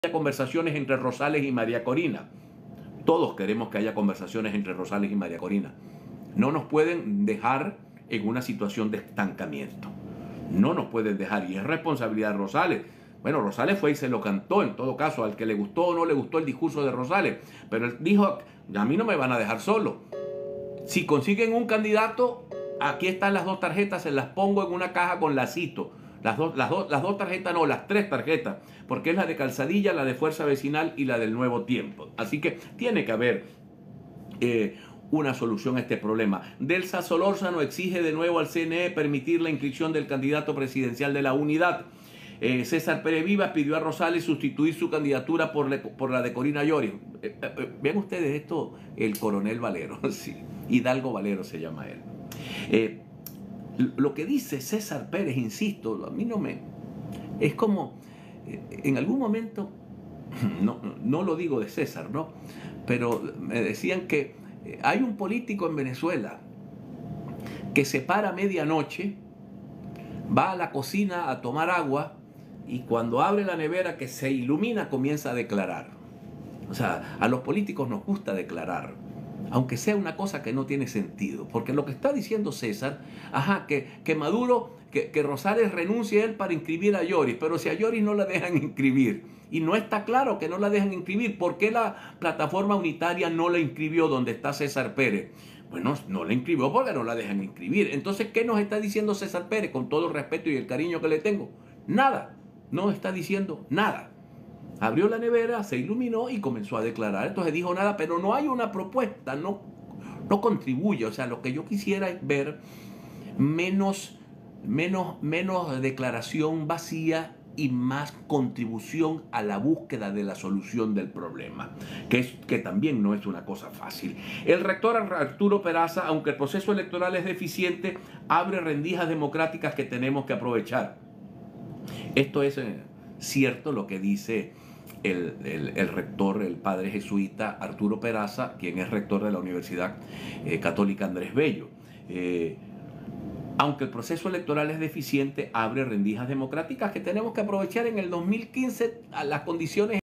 Estamos estancados.
la carta de renuncia de González Urrutia, María Corina y Rosales deben hablar, Maduro promulgó la ley del esequibo, no podemos quedarnos sin candidato.